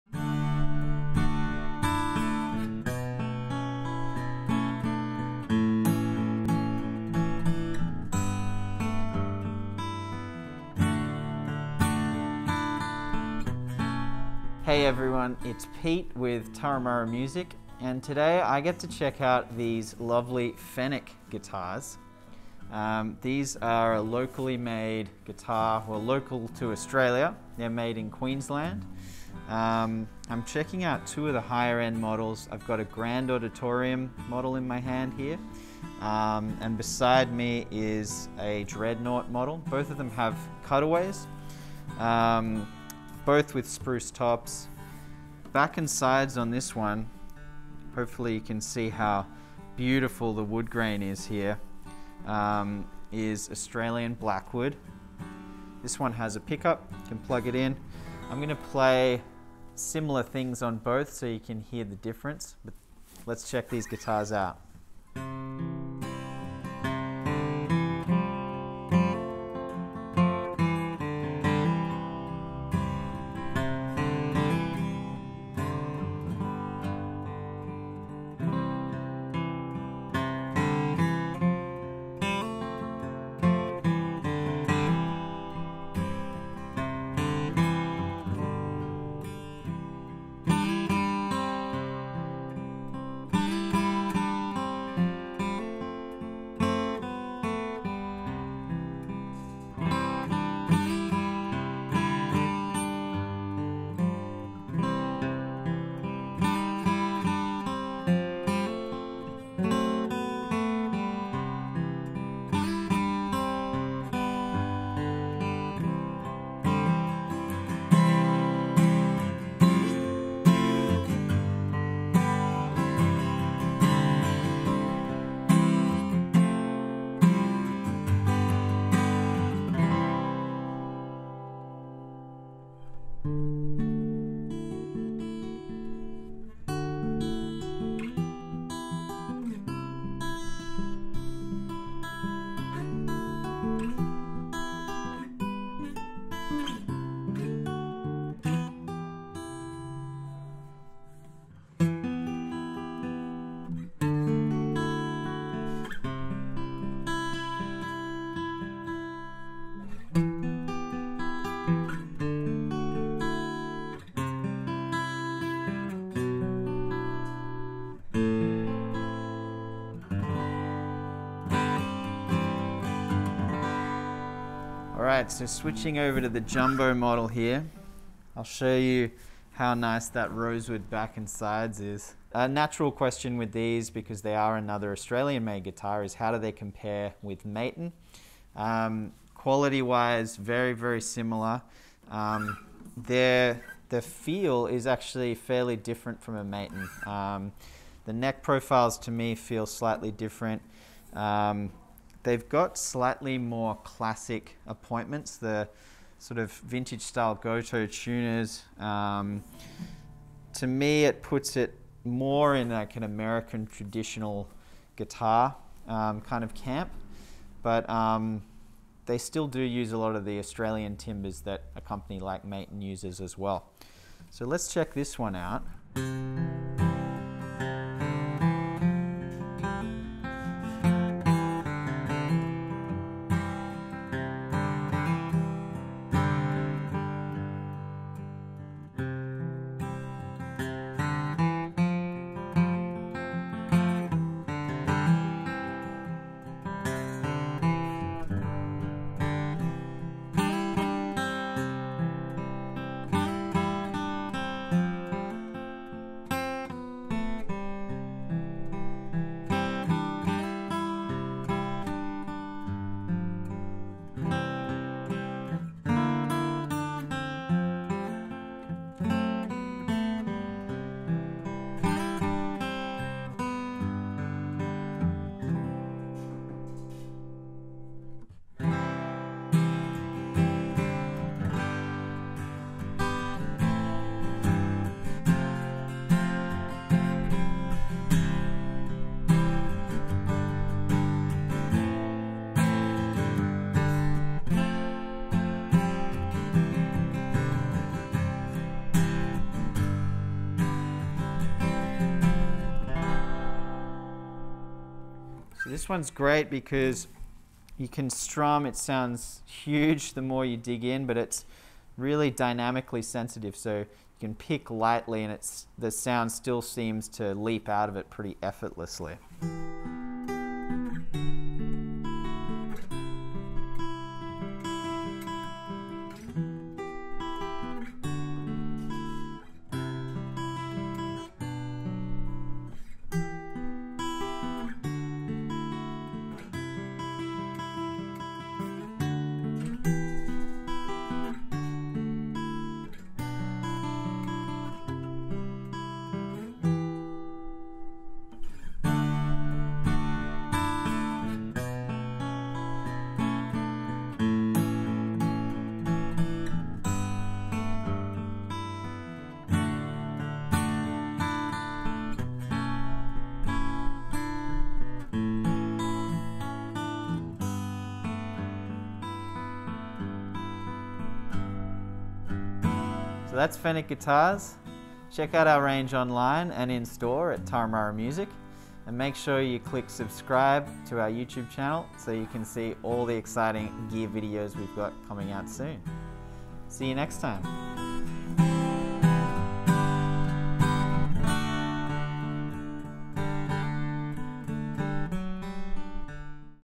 Fenech Guitars VT Pro – First Look at Turra Music
Pete samples some of the Gold Coast’s finest top shelf acoustics – Fenech Guitars’ VT Pro Grand Auditorium and VT Pro D78C.

Born from a passion for Music, Surfing and Art, Aaron Fenech officially launched Fenech Guitars in 2016 after many years of honing his craft by studying side by side some of Australia’s most revered master luthiers. Known for their ‘tone and volume’, Fenech Guitars are quickly gaining recognition as some of the best guitars made in Australia.

 Timestamps:
  0:00 – Intro
  0:15 – Overview
  1:05 – VT Pro Grand Auditorium
  3:02 – Compared to Maton?
  3:44 – VT Pro D78C
  6:13 – Closing

What are we using?